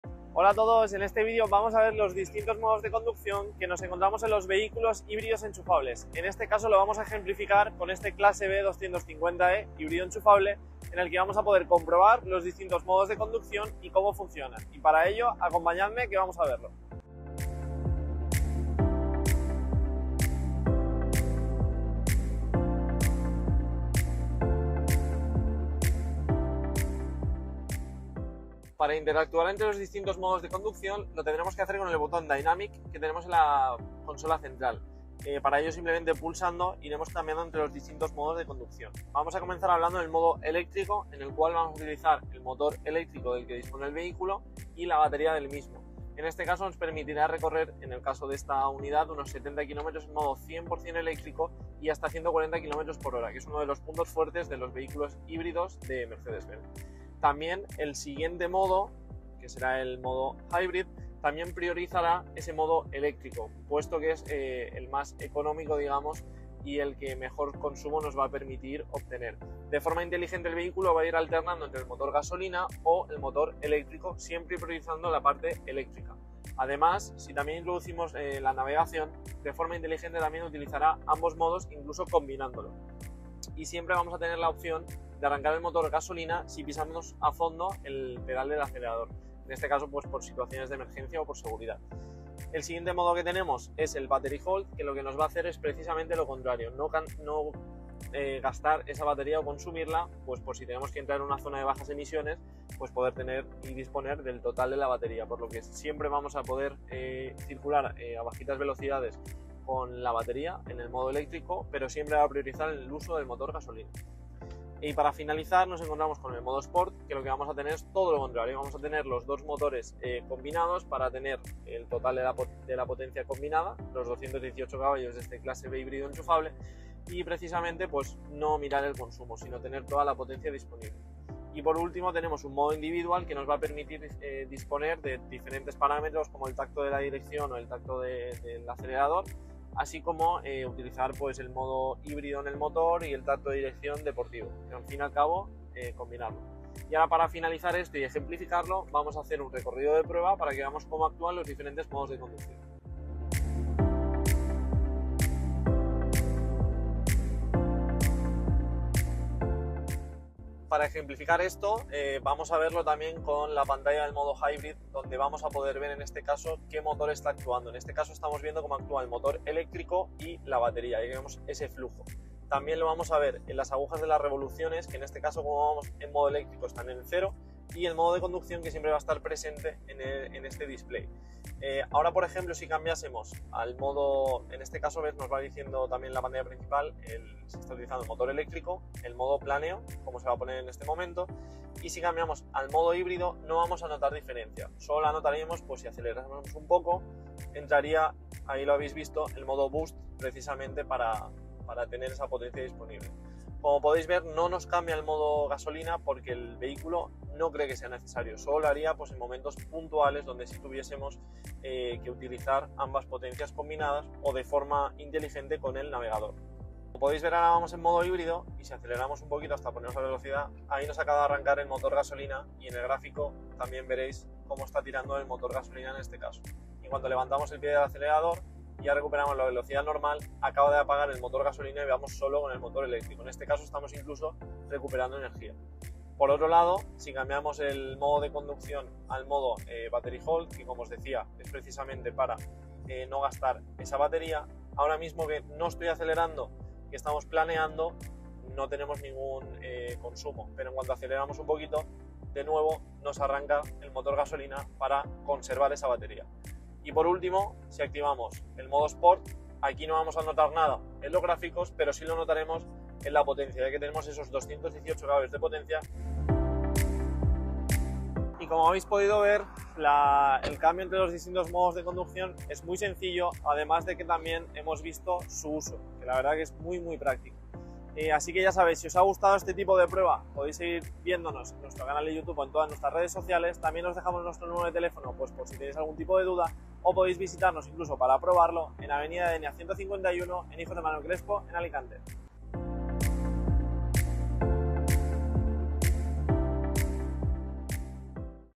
⚡ Modos de Conducción en un Híbrido Enchufable 🔋
Nuestro compañero, Alejandro Gil, nos explica los 𝐦𝐨𝐝𝐨𝐬 𝐝𝐞 𝐜𝐨𝐧𝐝𝐮𝐜𝐜𝐢𝐨́𝐧 en un híbrido enchufable y hace un ejemplo de conducción con un Clase B 250 e 👌

Ven a ver y a probar nuestros vehículos en nuestras instalaciones en Hijos de M. Crespo en Avenida de Denia 151 en Alicante 📍